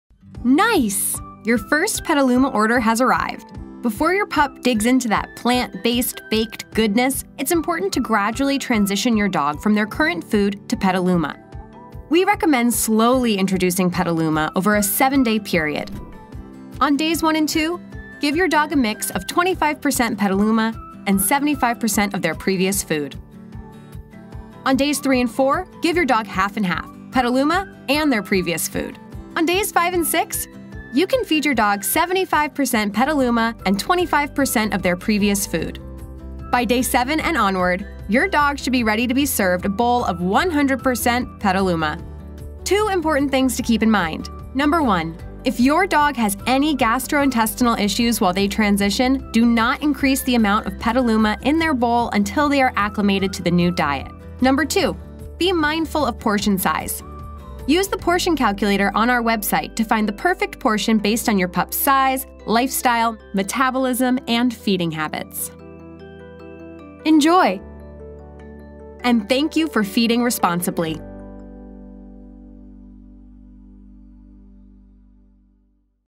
Transition guide for Petaluma dog food
Thanks for ordering Petaluma! Introducing your dog to a new food should happen slowly. Watch this video to learn how to successfully introduce and switchover to Petaluma from your dog's current diet.